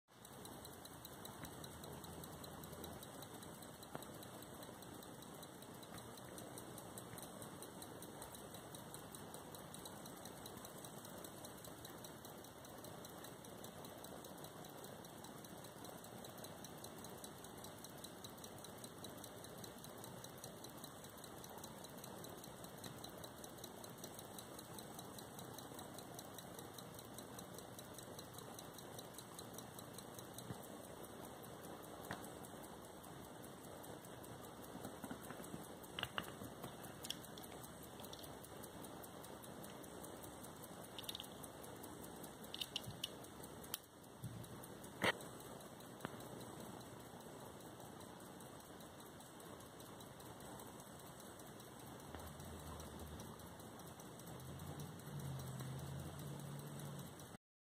Hamilton Railroad 21 Jewels Model 992 Silver Filled (SOLD OUT )
Hamilton Railroad Silver Filled 21 Jewels open face case  American made pocket watch
Circa : 1903 , it has 115 years old
Diameter = 48 mm , or 16 size
White porcelain dial that is signed Hamilton . The dial has Arabic numerals , blue steel hands , and subsidiary seconds . The watch has screw bezels from front , and back .
21 Jewels high grade movement . The watch is crown winding , and has lever - set movement . The movement is signed
Hamilton watch co .
LANCASTER . PA
ADJUSTED 5 positions
Model 992
Serial # 1596167
Double Roller
The nickel case is signed
Regal B & B
5993093